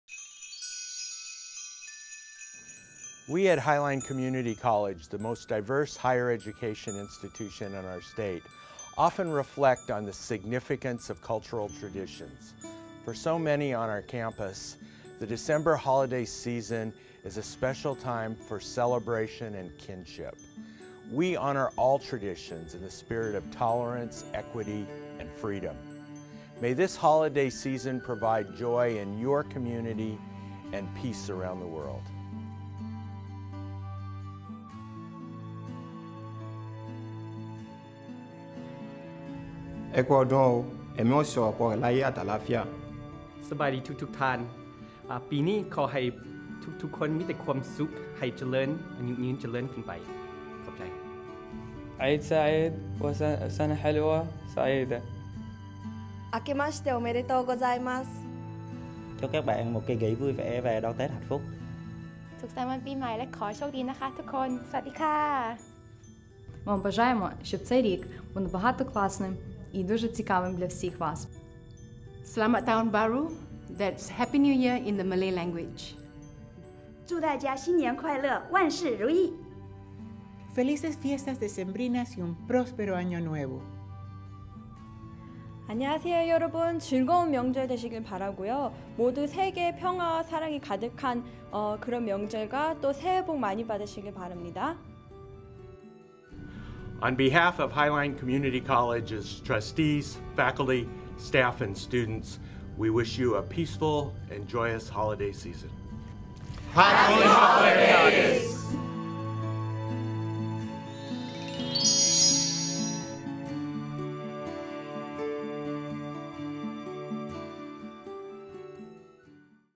Happy Holidays from Highline Community College
Highline Community College's Trustees, faculty, staff and students wish you a peaceful and joyous Holiday Season.